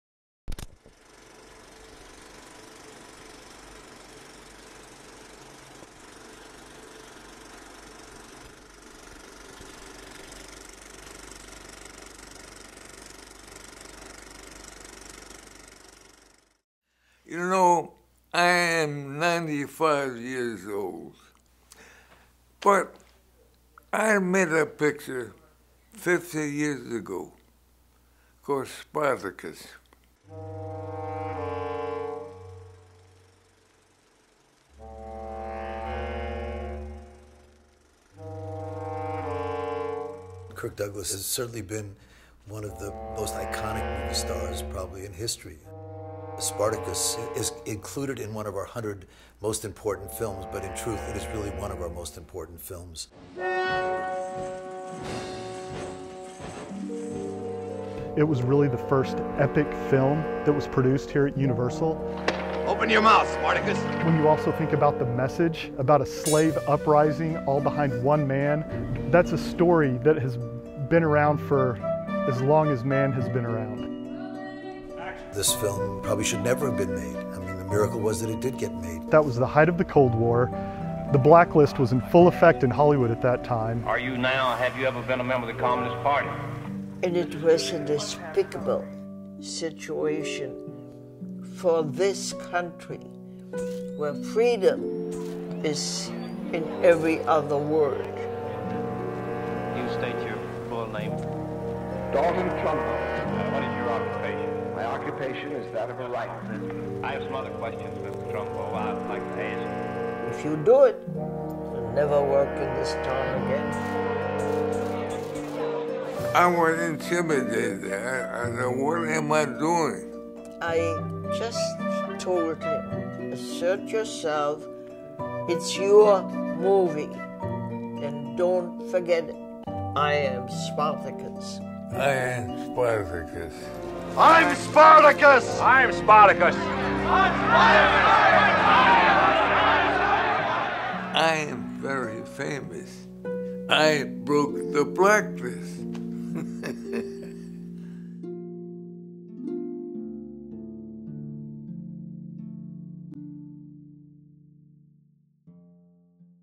Kirk Douglas's I AM SPARTACUS!
"This film probably should never have been made. The miracle was that it did get made."

The award-winning classic film Spartacus (1960) is remarkable in many ways: It was the most expensive movie ever made at the time and it was the first movie of an epic scope produced at Universal Studios. Most importantly, Kirk Douglas, its producer and star, insisted on giving public writing credit to blacklisted screenwriter Dalton Trumbo. Risking his own film and career, Douglas effectively ended the Hollywood blacklist.

Here Douglas, his wife Anne, Universal Studios President Ron Meyer, and Jeff Pirtle, archive director for NBC Universal, talk about the movie that changed it all.